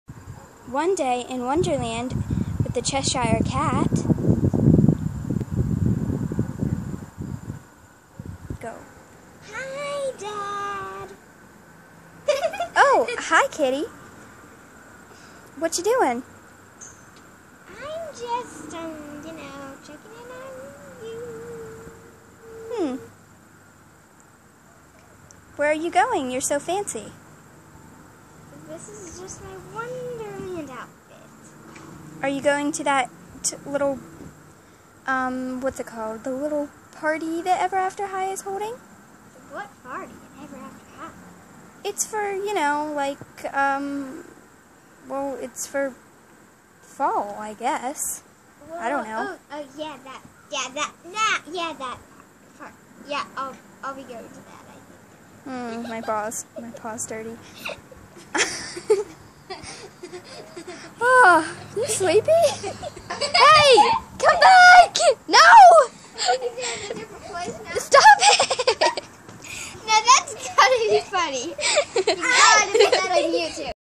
EAH Kitty Cheshire and The Cheshire Cat
Hey guys, my sister did the voice of Kitty Cheshire,and I did the voice of The Cheshire Cat (which is my cat).